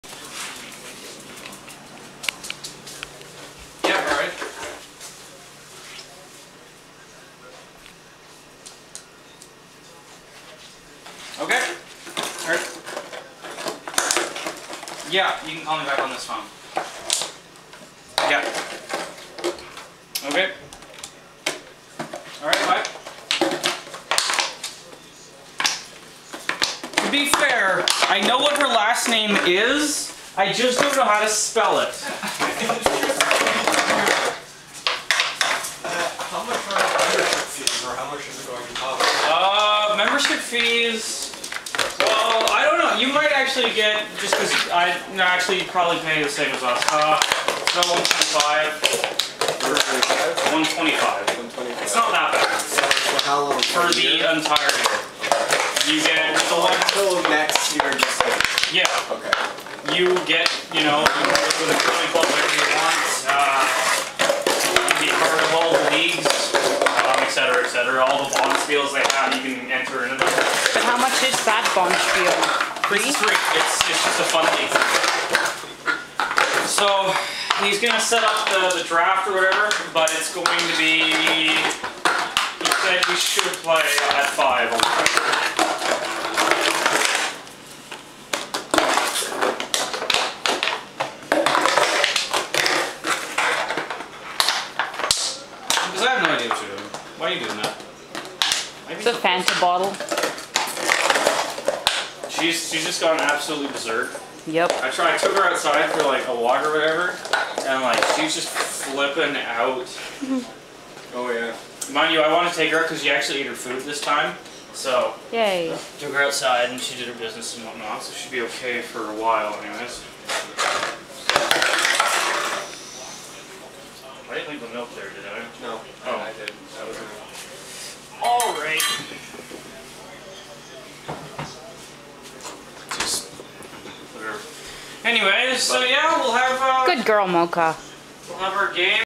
Moka playing with a Pepsi bottle
Moka is half Yorkie and half ???. She's smart yet stubborn, so she'll always know what you want her to do, but she'll only do it if she feels like it.